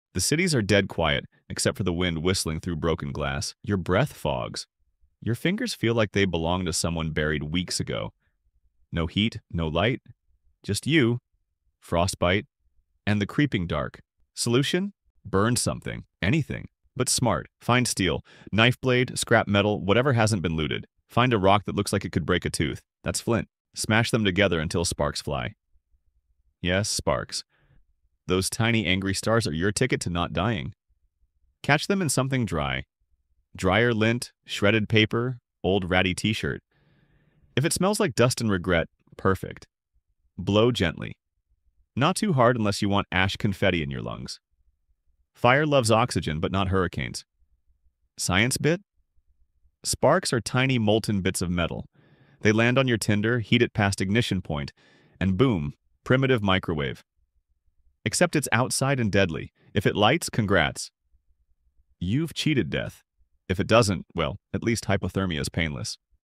How to Create Fire Without Matches in a Post-Apocalyptic World
No matches. No lighter. No problem. In this DIY survival science guide, learn how to create fire using simple tools and proven techniques that could save your life in an emergency. Perfect for post-apocalyptic survival, wilderness camping, or prepping skills.

For educational and entertainment purposes only. Always use caution with fire.

What you’ll learn in this video:

How to start fire using basic chemistry and friction

Using steel wool, batteries, and science to spark flames

Flint, rocks, and natural tinder for traditional fire-making

Why fire is the most essential survival skill

This experiment shows how science and resourcefulness can keep you alive when technology fails.